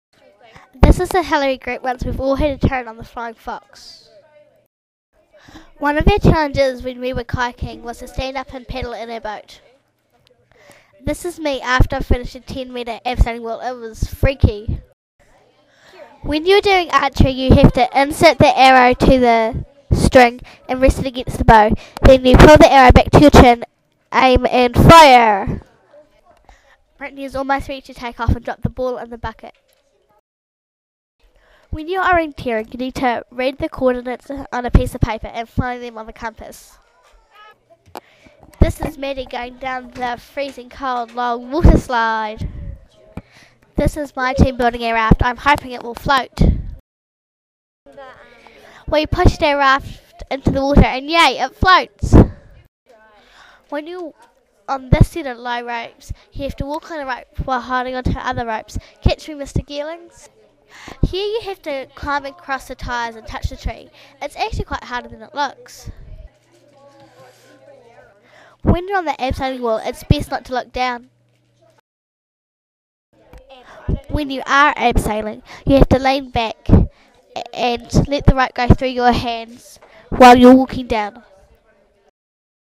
Gabby's x-factor x-pedition photostory
Gabby has created a photostory about our X-factor X-pedition to Camp Columba in Pukerau (Southland, NZ) in late March -we all had a great time!